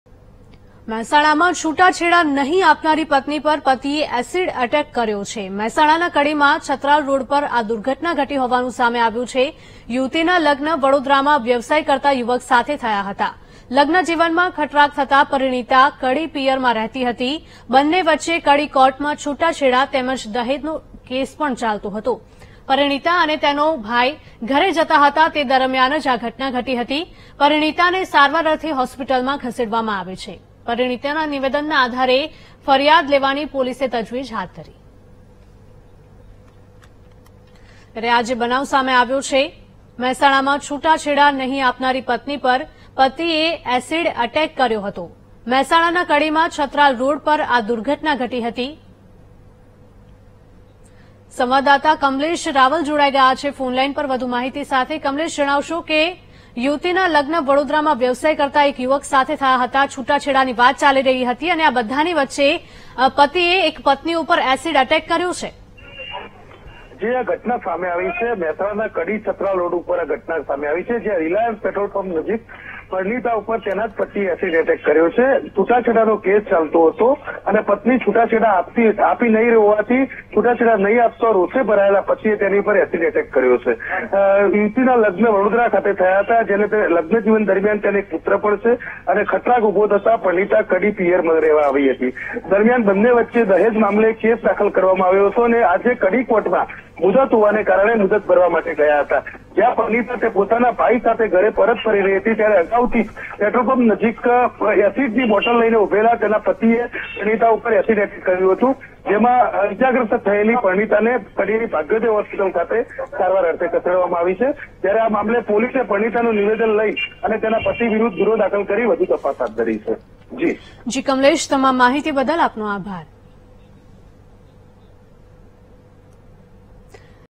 Mehsana માં છૂટાછેડા નહીં આપનારી પત્ની પર પતિએ કર્યો એસિડ અટેક, કડીમાં છત્રાલ રોડ પર બની ઘટના | VTV
Mehsana માં છૂટાછેડા નહીં આપનારી પત્ની પર પતિએ કર્યો એસિડ અટેક, કડીમાં છત્રાલ રોડ પર બની ઘટના | VTV Gujarati